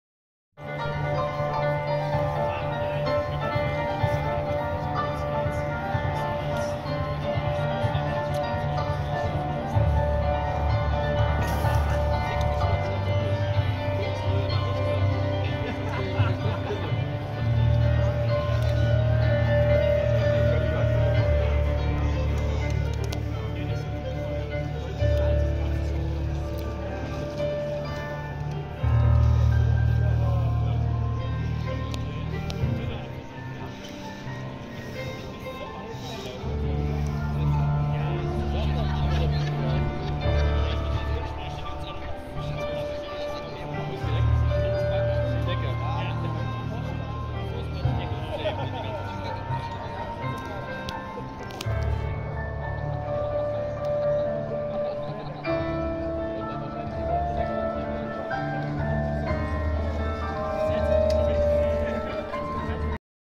vor Rammstein in Dresden am 16.05.2024
Geocaching mit KoerniDD

Wir waren als Zaungäste bei Rammstein.
hatte den Vorteil, man konnte die Musik noch laut genug hören und gemütlich ein Bierchen dazu trinken und vor allem konnte man sich dabei noch unterhalten.

Vor Rammstein, es gab keine Vorband, hier wurden nur auf dem Klavier einige Lieder von Rammstein gespielt.
Nur mal ein kleiner Rundblick auf dem Platz vor dem Konzertgelände.

KoerniDD 2024